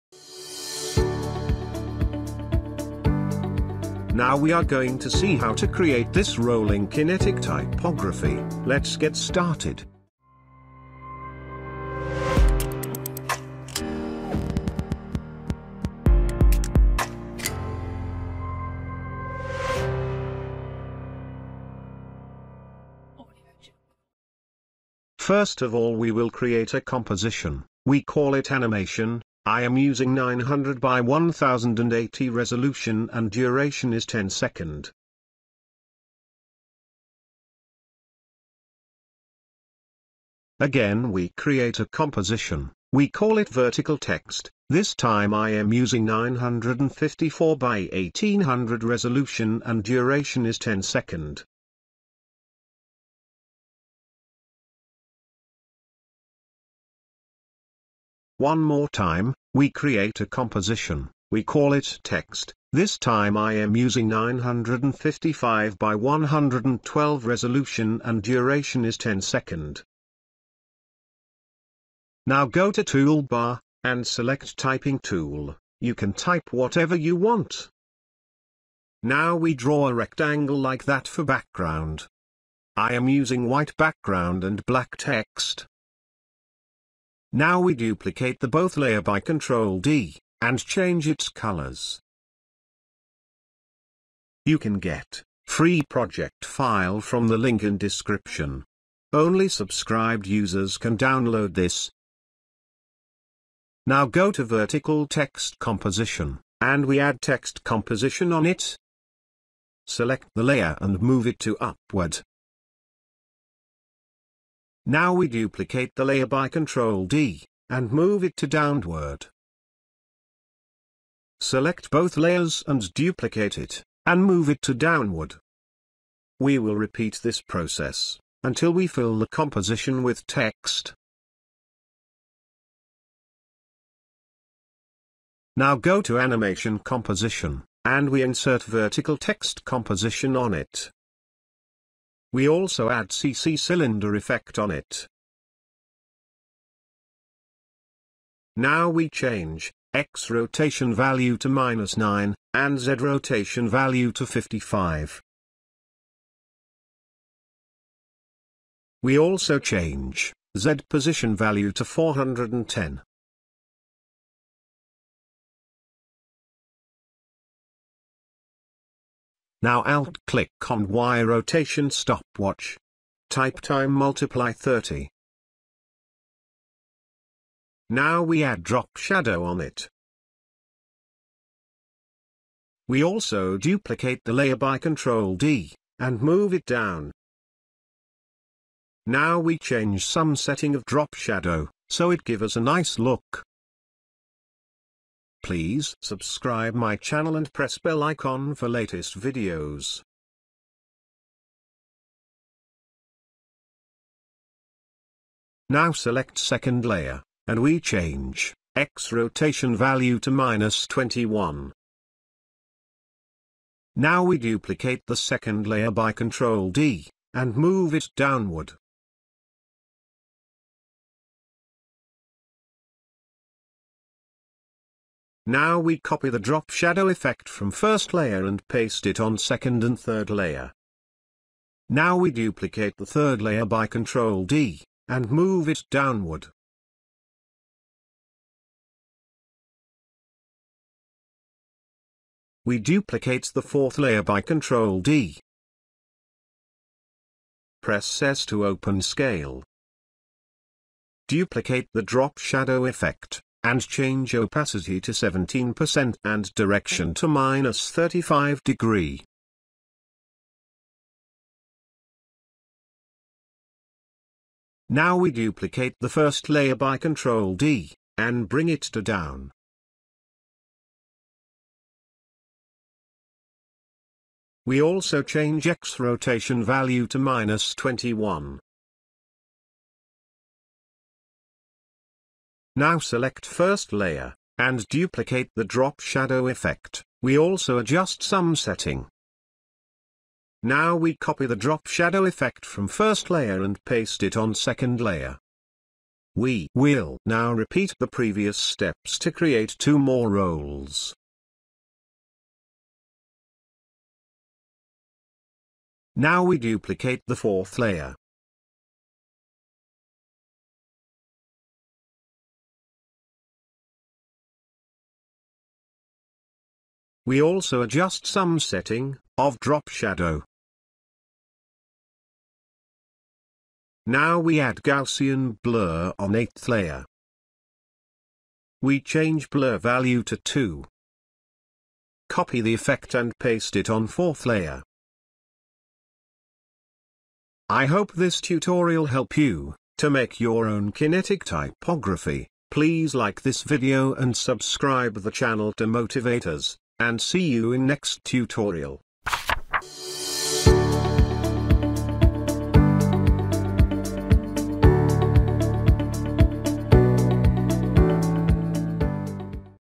Rolling Kinetic Typography | After Effects Tutorial | Free Project File
🙏 Hello Friends, In this Video we will create " Rolling Kinetic Typography In After Effects 2020 "  .